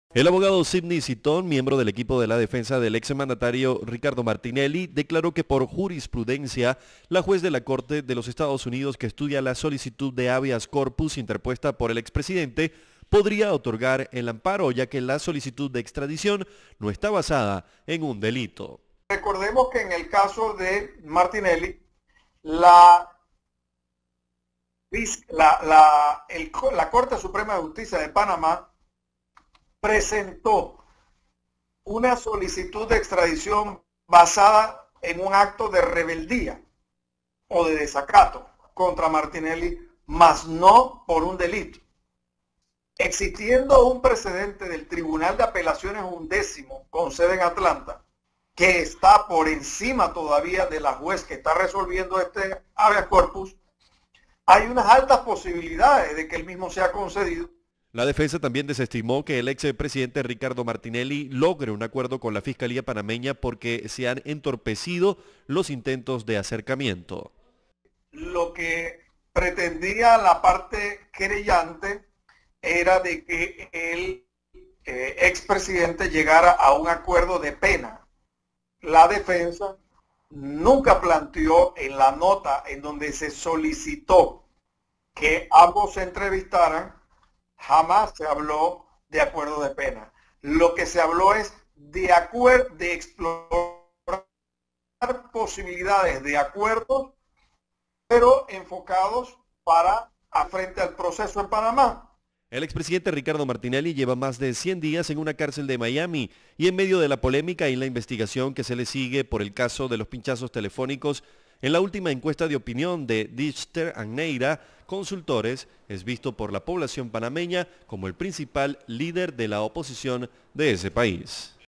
Expertos intentan nuevas acciones en defensa de expresidente Martinelli
La Defensa del expresidente de Panamá Ricardo Martinelli, estima que Juez de Estados Unidos resuelva en los próximos días la solicitud de Habeas Corpus que la defensa introdujo como amparo para evitar su  extradición.

El abogado Sídney Sittón miembro del equipo de la defensa del ex mandatario Martinelli declaró que por jurisprudencia la juez de la corte de Estados Unidos que estudia la solicitud de Habeas Corpus interpuesta por el expresidente, podría otorgar el amparo,  ya que la solicitud de extradición no está basada en un delito.

La defensa también desestimó que el ex presidente Ricardo Martinelli logre un acuerdo con la fiscalía panameña porque se han entorpecido los intentos de acercamiento.

El expresidente Ricardo Martinelli lleva ya 100 días en una cárcel en Miami y en medio de la polémica y la investigación que se le sigue por el caso de los pinchazos telefónicos en la última encuesta de opinión de Dichter & Neira consultores, es visto por la población panameña como el principal líder de la oposición de ese país.